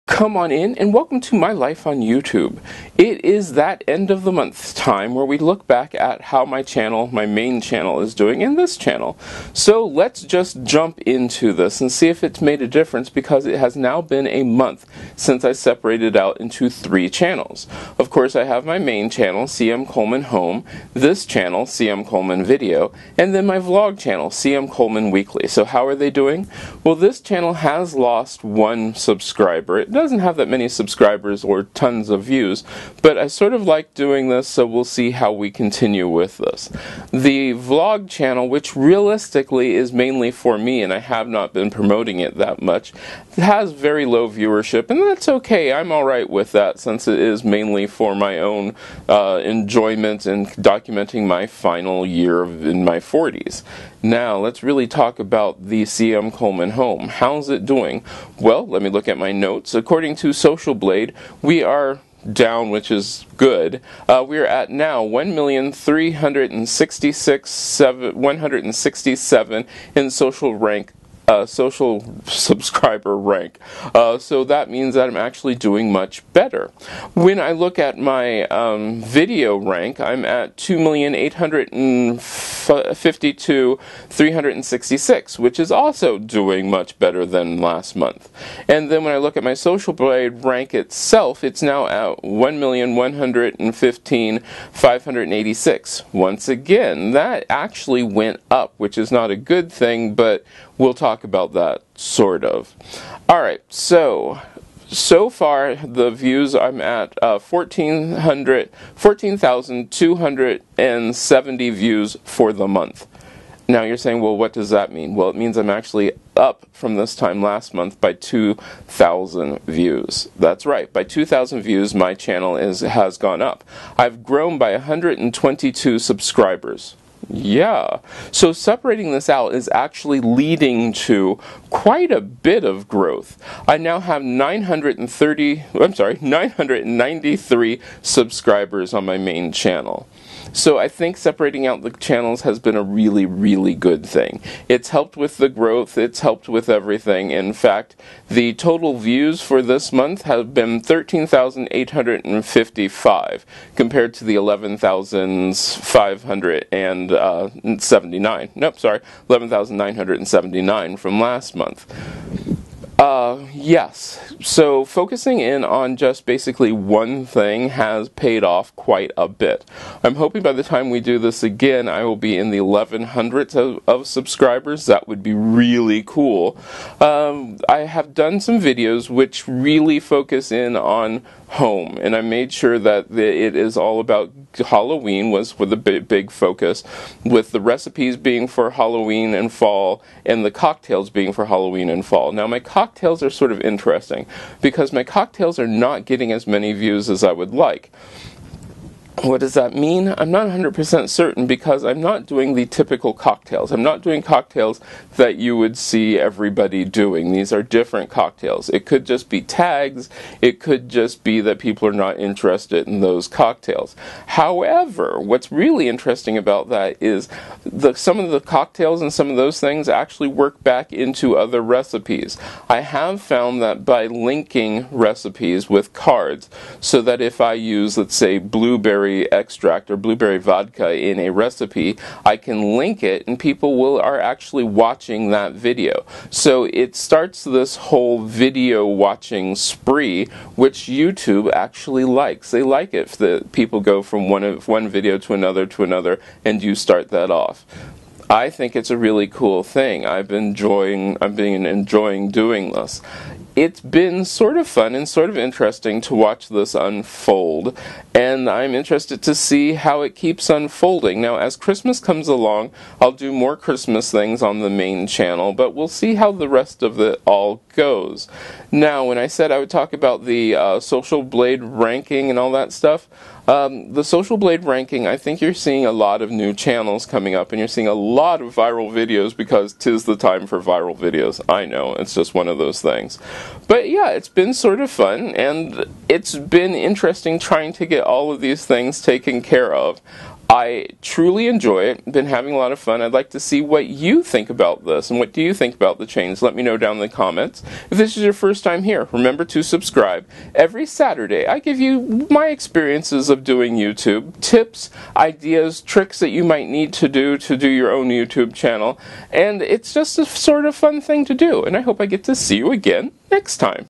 October 2016 Channel Check Up My life On YouTube
It has been 1 month since I seprated out the channels. How are they doing.  I let you know. I was sort of surprised at the results.

PRODUCTION:
AudioBlocks Member referral:

KITCHEN SET
Lights
Camera
Microphone
Daylight Fluorescent
SELS LED Tube lights 18 Watts 4 Foot

OFFICE SET
Camera